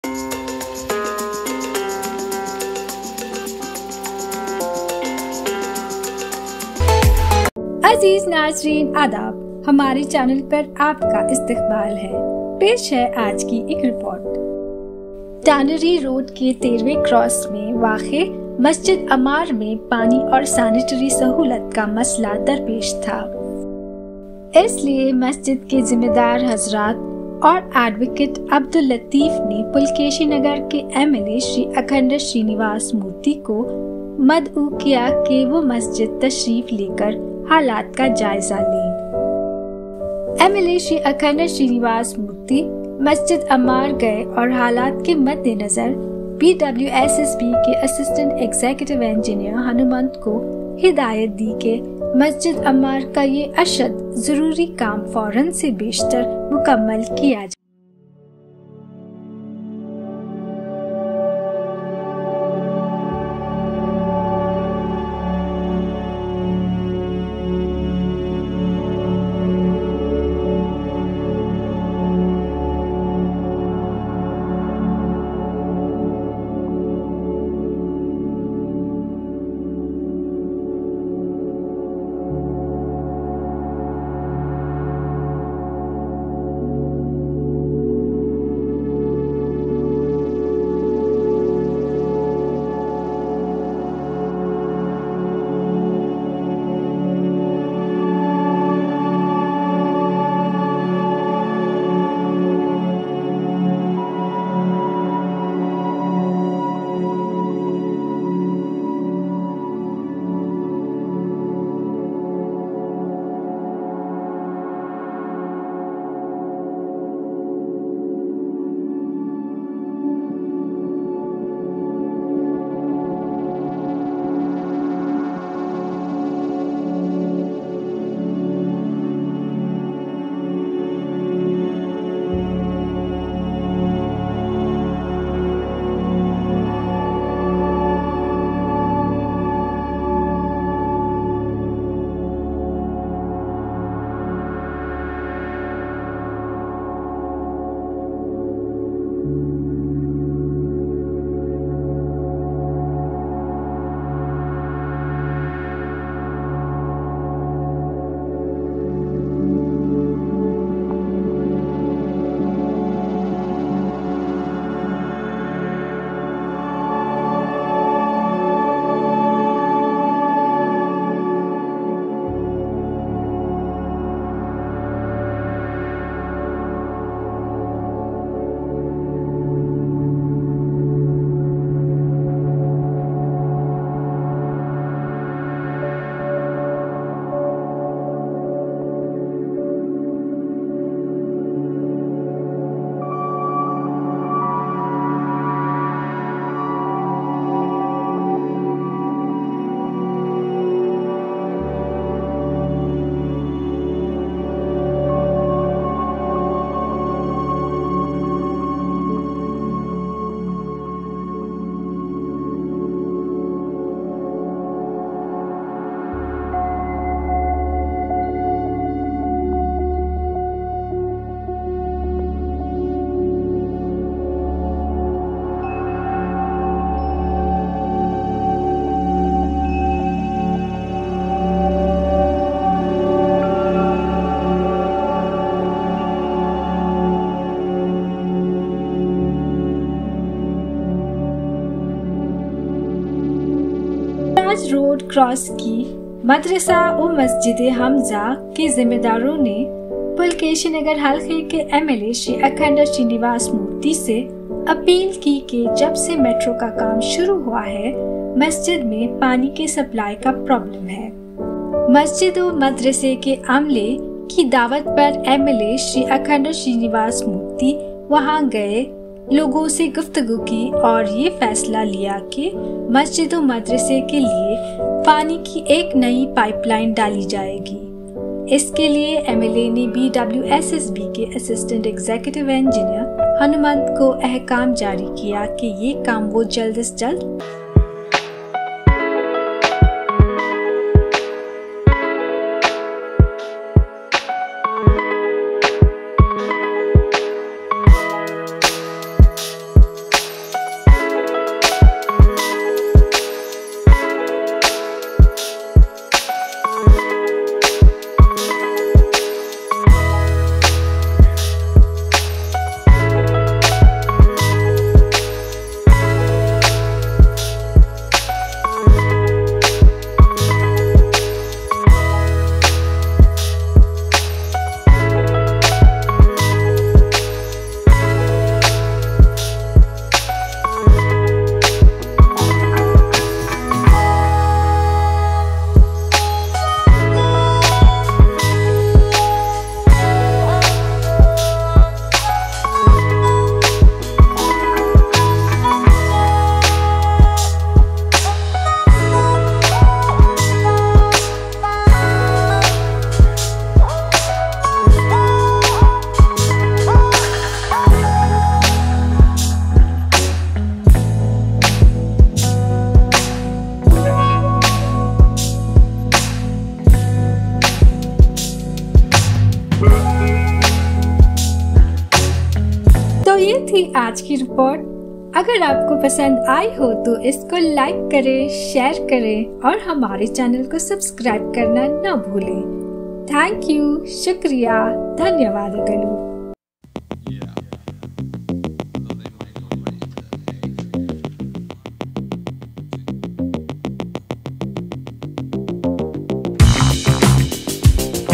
MLA Akhanda Srinivas Murthy promises to provide basic amenities at the Mosque!
In this video, we discuss the recent development initiative taken by our MLA Akhand Srinivasmurthy to install sanitary and water pipelines in Masjid Ammar mosque and Madrasa o Masjide Hamza. It was causing inconvenience to worshippers, especially during peak hours. Therefore, the MLA decided to take immediate action to ensure proper hygiene in the mosque.